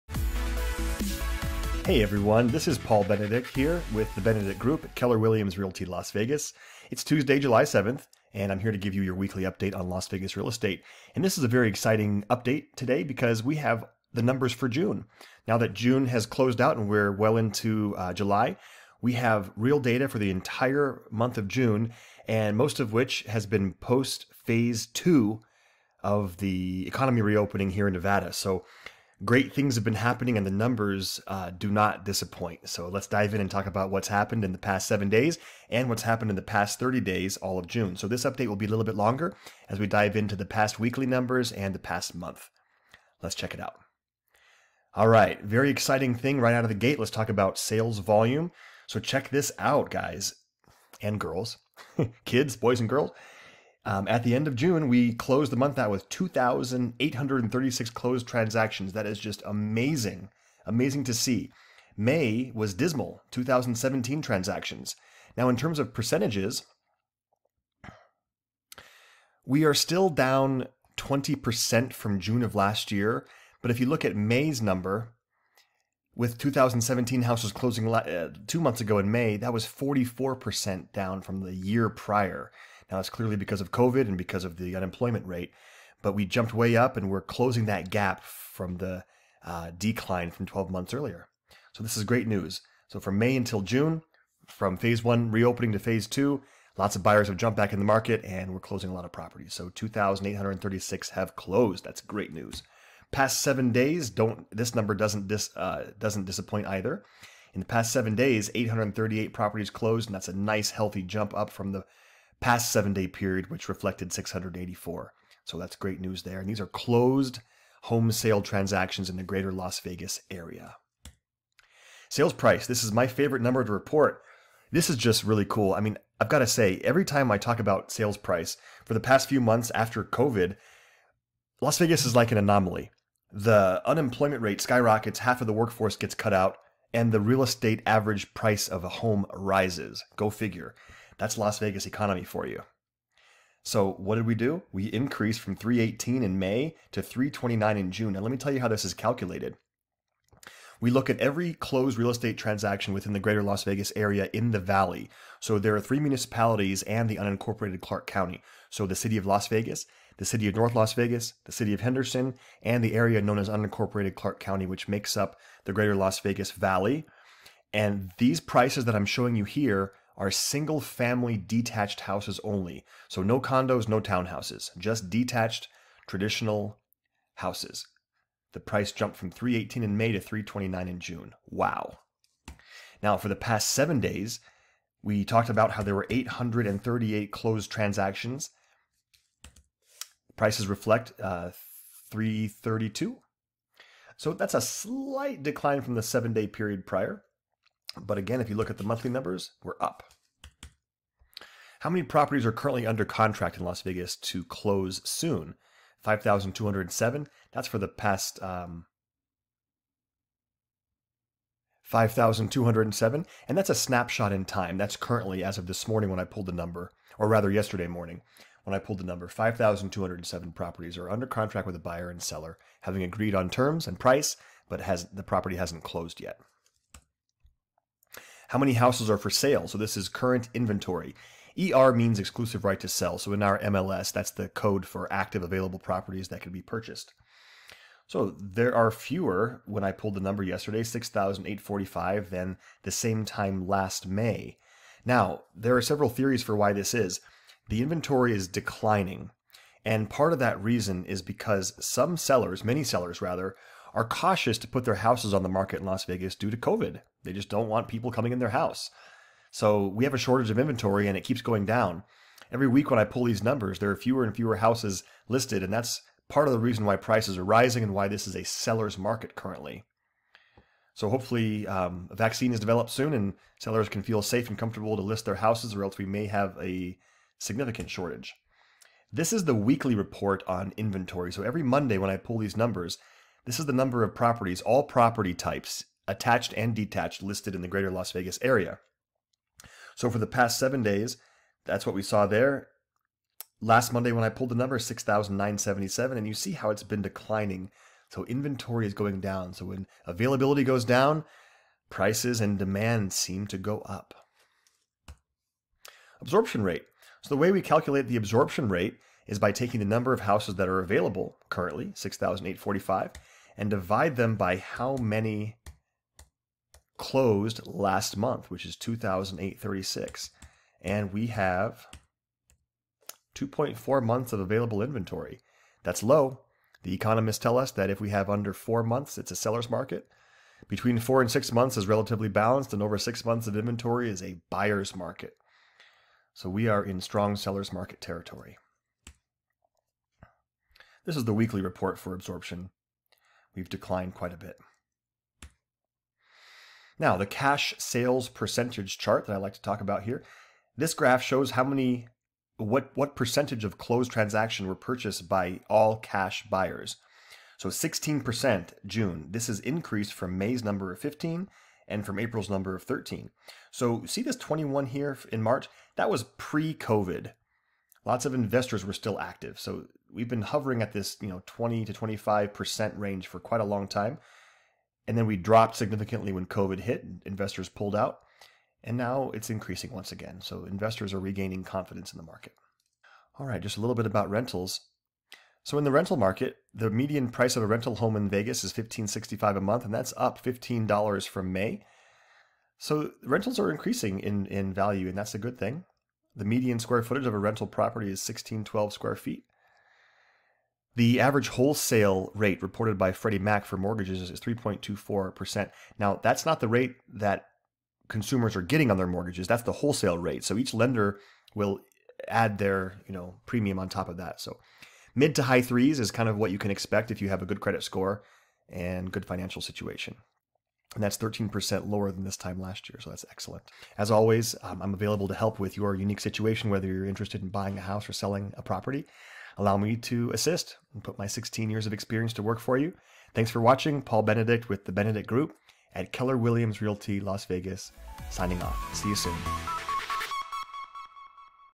Las Vegas Real Estate Market Update 7-7-20
Paul Benedict, Broker Salesperson with Keller Williams Realty Las Vegas describes the current state of Las Vegas Real Estate.